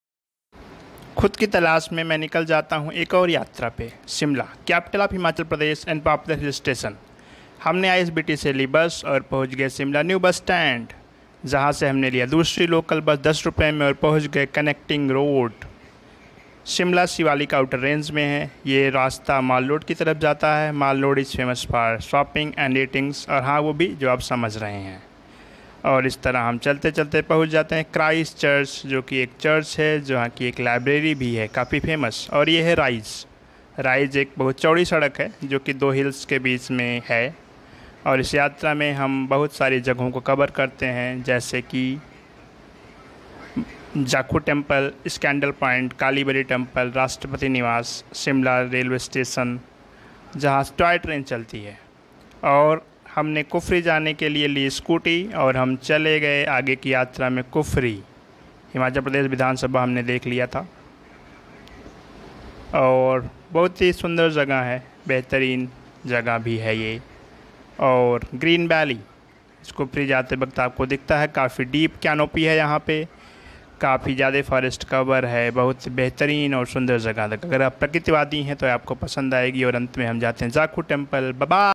Shimla diaries The Ridge#Mall Road Jakhu temple#Green valley
Shimla :is the capital and the largest city of the northern Indian state of Himachal Pradesh. In 1864, Shimla was declared as the summer capital of British India. After independence, the city became the capital of East Punjab and was later made the capital city of Himachal Pradesh. It is the principal commercial, cultural and educational centre of the state.
Food to try: Siddu
Places to visit
1.Mall road
2. The Ridge
3. Christ church
4. saddle point
5. Kalibari temple
6. Jakhu temple
7. Rastrapati niwas
8. Simla Railway station
9.The Green valley
10. Kufri.
 Places 1 to 9 u can walk if u love trekking and is young physically, for 9 and 10 you can rent taxi or bike